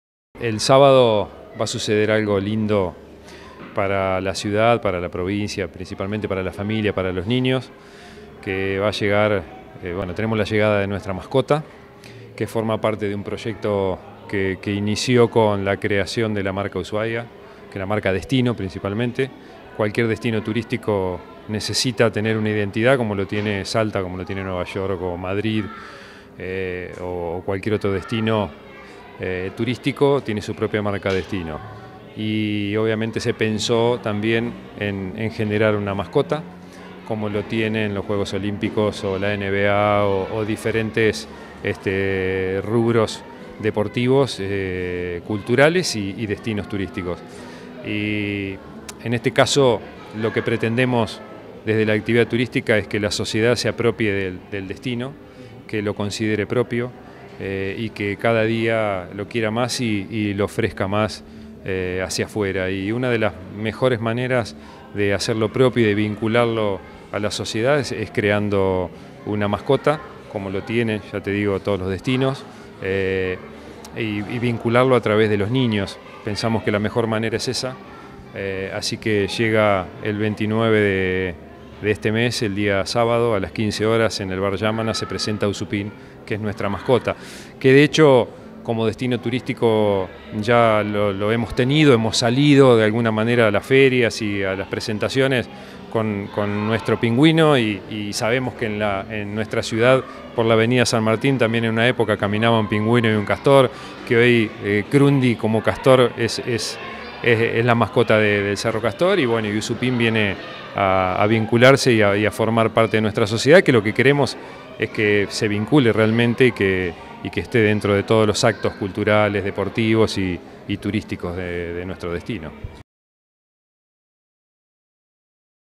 PRESENTAN A “USUPIN”, MASCOTA OFICIAL DE USHUAIA
De la mano de la Municipalidad de Ushuaia y de la Confederación Agencia de Desarrollo Ushuaia se presentará en un festival abierto al público a USUPIN, la primera mascota oficial de Ushuaia que tendrá el objetivo de transmitir las riquezas naturales y principales atractivos de la ciudad más austral del mundo, posicionando la experiencia única de visitarla en cada estación del año.